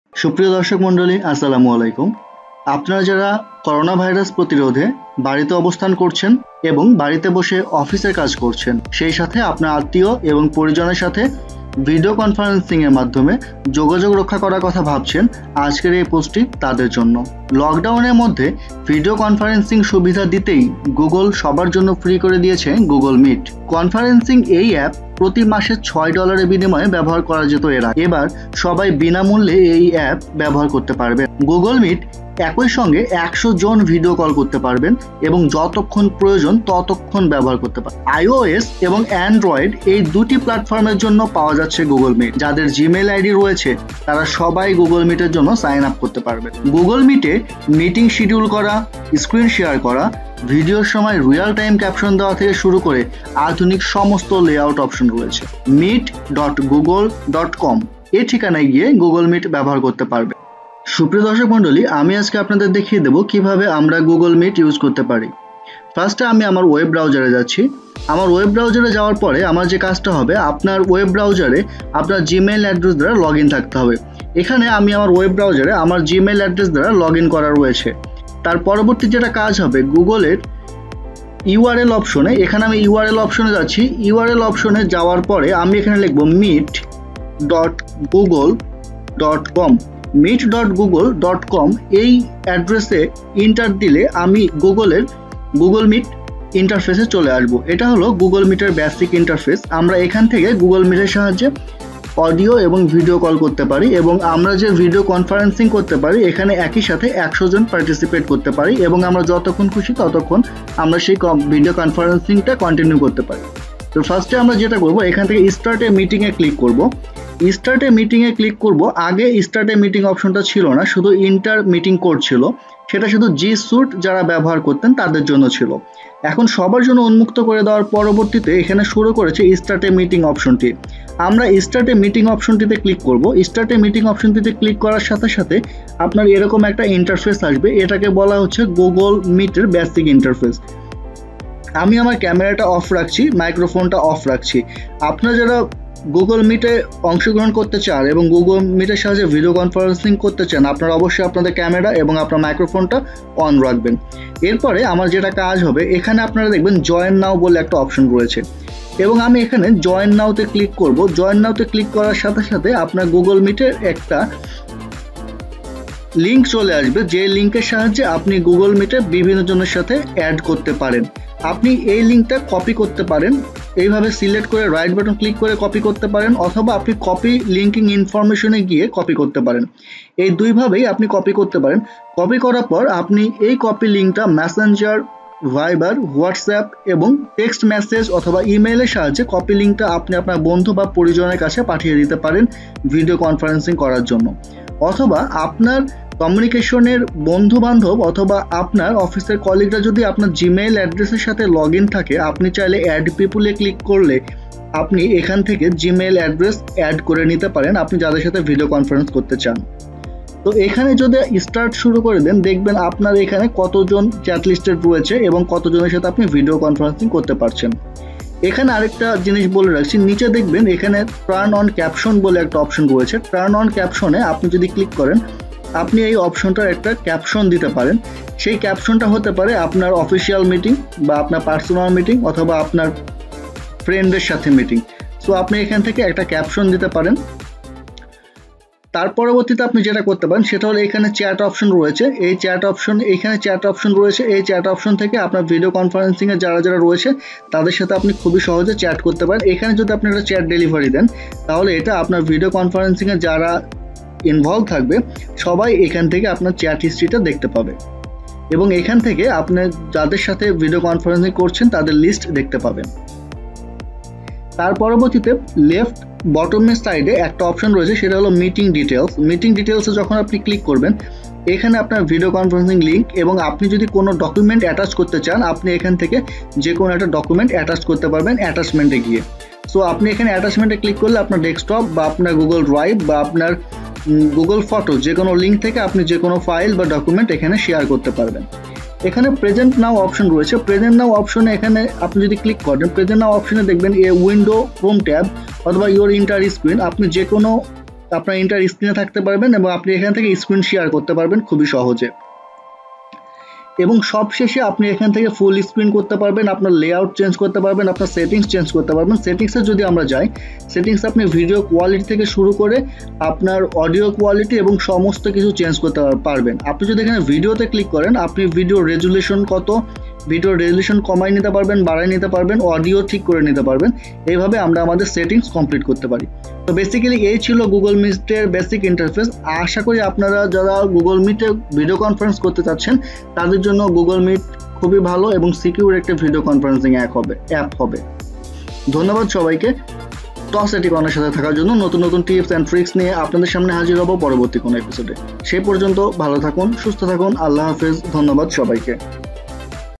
Google Meet এখন সবার জন্য বিনামূল্যে || How to Use Google Meet
In this video I will show you how latest launching news of google meet how to use google meet and of course how to use google meet in detailed tutorial. I will also show you how to start a call in google meet, how to join a call in google meet. What is google meet invitation link and how to share google meet invitation link with friends and college. Hopefully this video will help to to do video conferencing using google meet.

Thank you so much to watch TOS IT Corner.